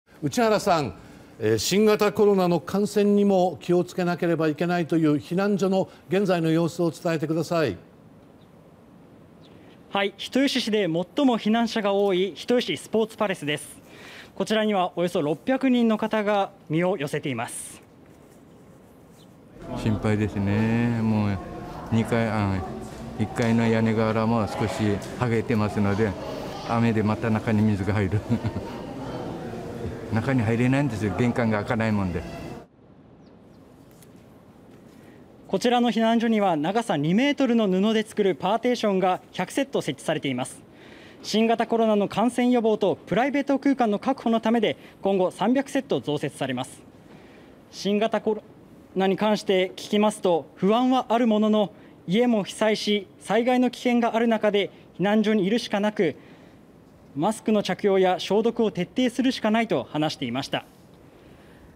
「心配です」コロナ禍の避難所に課題 熊本・人吉市(20/07/06)
熊本県では南部を流れる球磨川の氾濫などでこれまでに26人が死亡しました。市街地が広範囲に浸水した人吉市では11人が死亡しました。新型コロナウイルスの感染にも気を付けないといけない避難所の様子を人吉市の避難所から報告です。

　（内原健文アナウンサー報告）
　人吉市で最も避難者が多い人吉スポーツパレスには約600人の人が身を寄せています。避難所には長さ2メートルの布で作るパーテーションが100セット設置されています。新型コロナウイルスの感染予防とプライベート空間の確保のためで今後、300セット増設されます。新型コロナウイルスに関して聞くと、不安はあるものの、家も被災して災害の危険があるなかで避難所にいるしかなく、マスクの着用や消毒を徹底するしかないと話していました。6日からは日赤の救護所も開設され、健康面に不安を抱える人などの診察なども行われています。